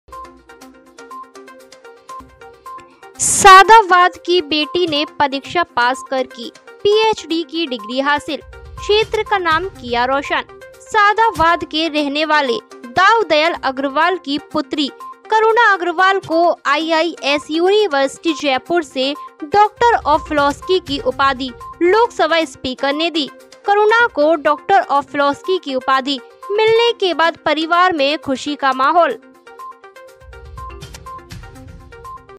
सादाबाद की बेटी ने परीक्षा पास कर की पी एच डी की डिग्री हासिल
सादाबाद एक बेटी पीएचडी की परीक्षा पास कर की पी एच डी डिग्री हासिल । क्षेत्र का नाम रोशन किया है सादाबाद की आने वाली रहने वाले दाऊ दयाल अग्रवाल की पुत्री करुणा अग्रवाल ने आईआईएस यूनिवर्सिटी जयपुर  से डॉक्टर ऑफ फिलोस्की की उपाधि लोकसभा स्पीकर  ने दी है। करुणा को डाक्टर ऑफ फिलोसकी की उपाधि मिलने के बाद परिवार में खुशी का माहौल है